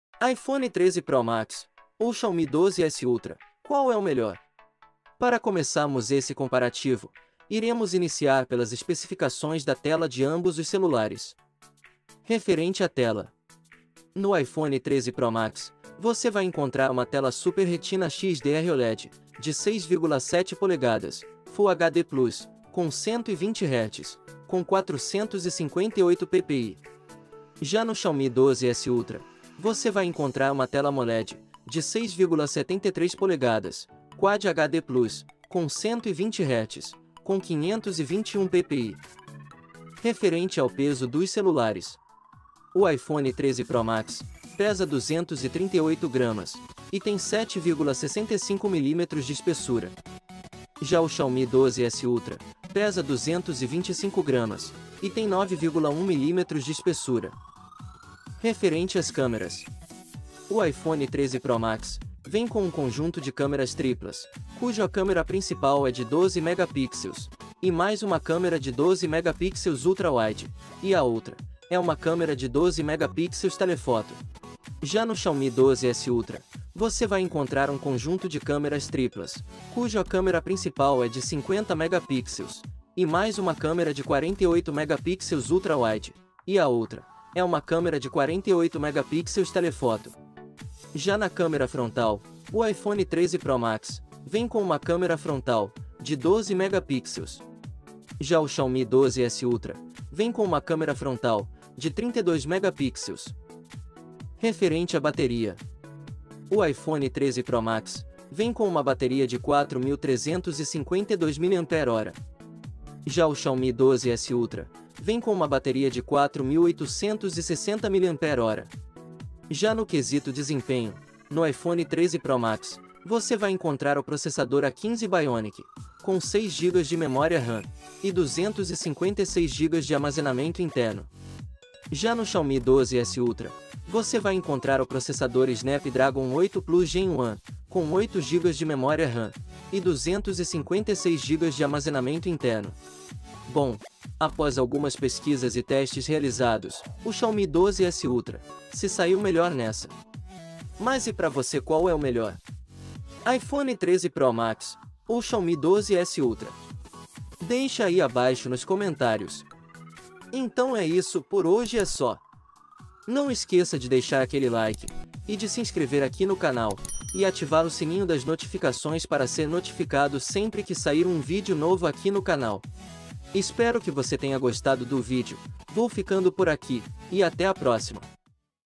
iPhone 13 Pro Max vs Xiaomi 12S Ultra Qual é o MELHOR? (Comparativo)
iPhone 13 Pro Max vs Xiaomi 12S Ultra Qual é o MELHOR?

Está querendo trocar de celular mais está em dúvida entre iPhone 13 Pro Max ou Xiaomi 12S Ultra?

Fique aqui comigo, assista esse vídeo até o final e descubra hoje mesmo, qual é o melhor para você, entre iPhone 13 Pro Max x Xiaomi 12S Ultra!

Nesse vídeo comparativo busquei passar todas as informações importantes sobre o iPhone 13 Pro Max e do Xiaomi 12S Ultra

Nesse vídeo abordei assuntos do tipo:

🟢Tela
🟢Espessura
🟢Câmeras
🟢Bateria

Se você quer de fato saber qual é o melhor iPhone 13 Pro Max ou Xiaomi 12S Ultra você precisa ver esse vídeo URGENTE!

⏱ NÃO PERCA MAIS TEMPO ASSISTA AGORA MESMO ESSE VÍDEO REVIEW/COMPARATIVO Entre Apple iPhone 13 Pro Max ou Xiaomi 12S Ultra
E deixa aqui abaixo nos comentários a sua opinião de qual é o MELHOR entre iPhone 13 Pro Max ou Xiaomi 12S Ultra!